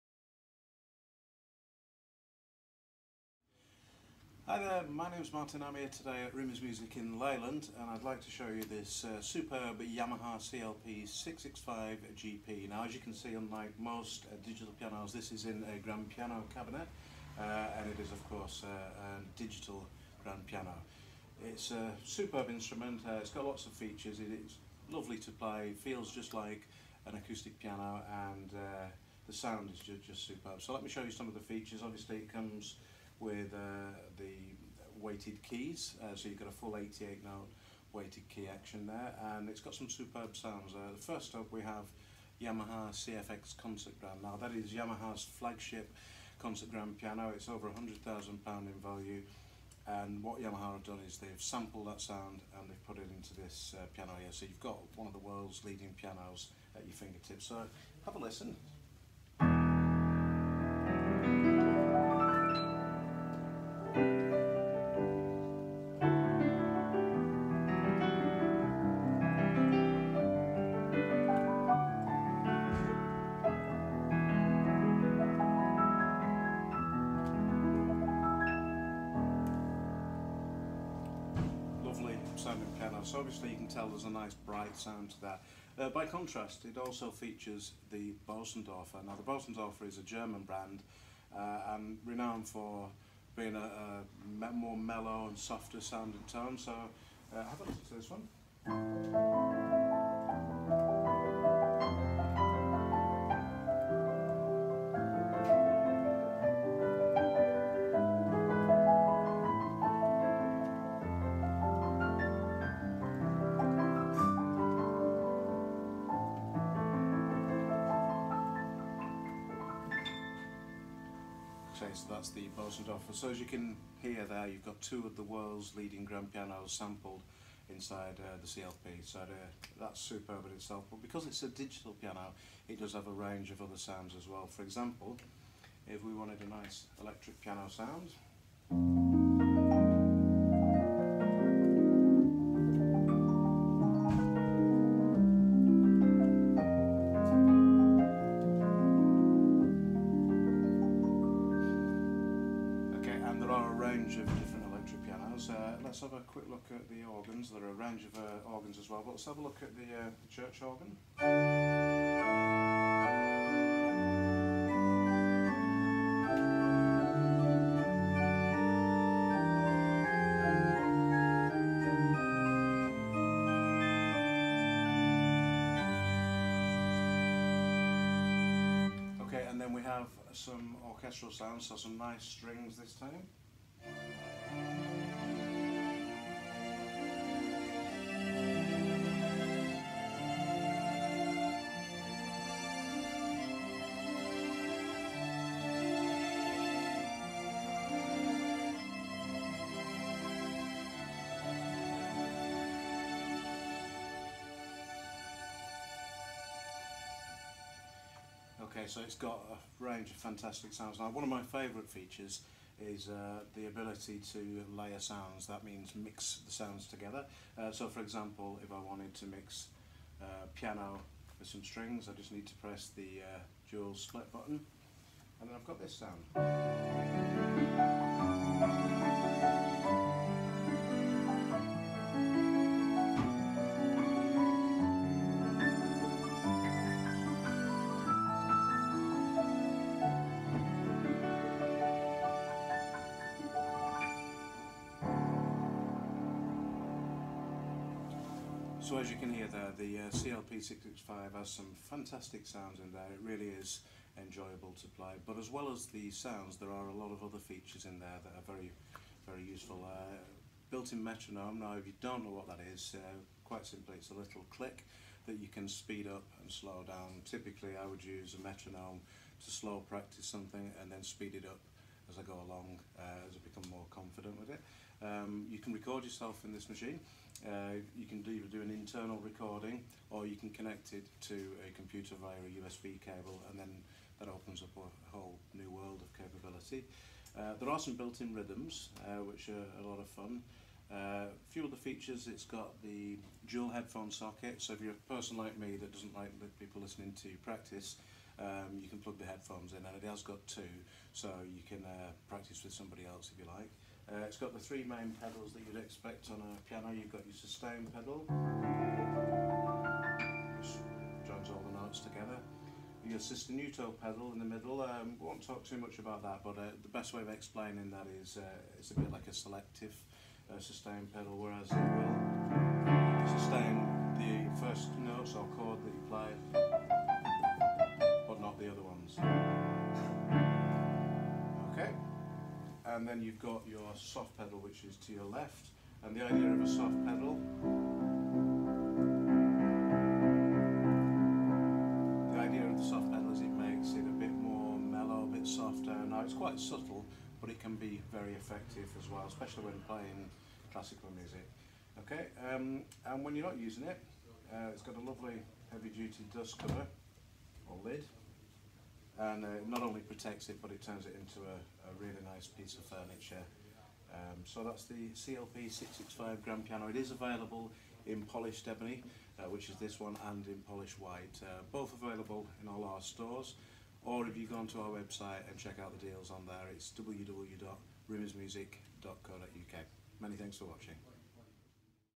Yamaha CLP-665GP Clavinova - Rimmers Music
Here we have Martin, over in our Leyland Store, demonstrating the Yamaha CLP665. The Yamaha CLP 665 Digital Grand Piano has impressive aesthetics and expressive playability for the advanced musician.